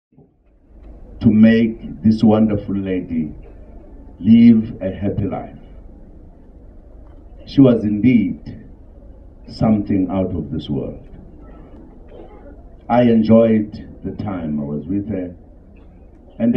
President Jacob Zuma at Ontlametse's funeral
Zuma speaks at Ontlametse Phalatse's funeral